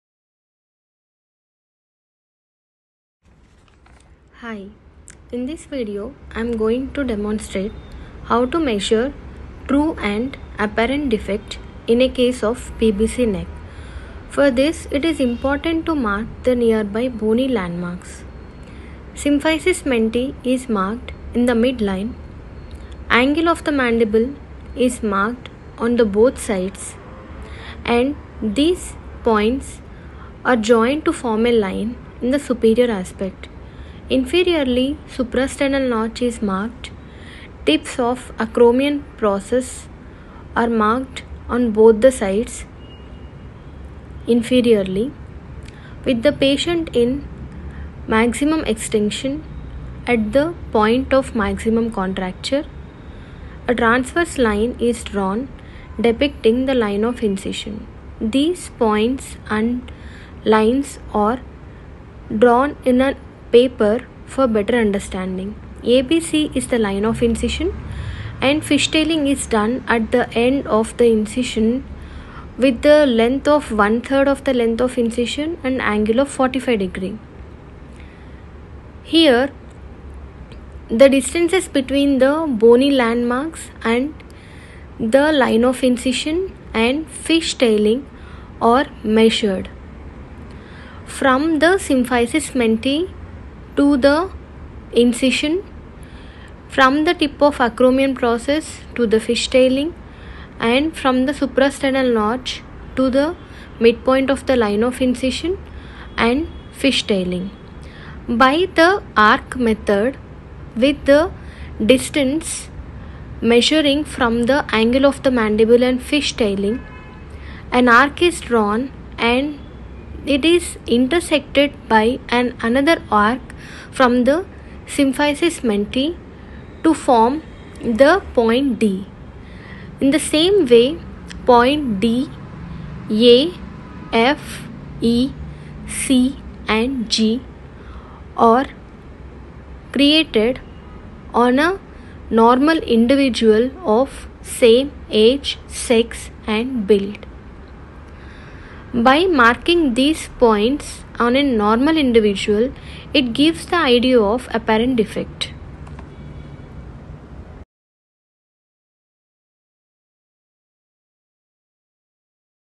True & Apparent defect
This video discusses True and Apparent defects for post burn contractures.